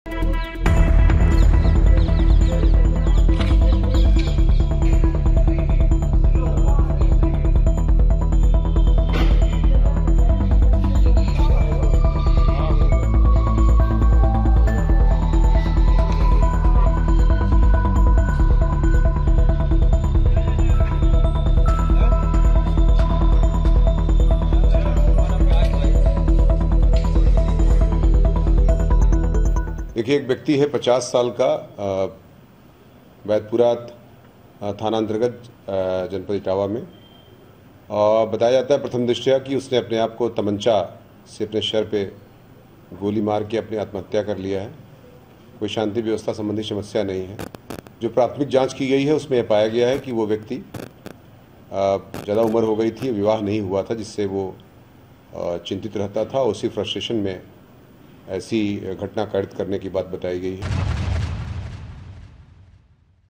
शादी न होने से परेशान युवक ने खुद को मारी गोली, मामले की जांच में जुटी Police | UP News | Aaj Tak
इटावा में एक व्यक्ति ने शादी न होने से परेशान होकर गोली मारकर आत्महत्या कर ली। वैदपुरा थाना क्षेत्र के ग्राम कैलोखर कापूरा मामला है. घटना की सूचना पर पुलिस फोरेंसिक टीम ने घटना स्थल पर छानबीन की। शुरुआती जांच के मुताबिक मृतक व्यक्ति 50 साल का हो चुका था और शादी ना होने के कारण परेशान था.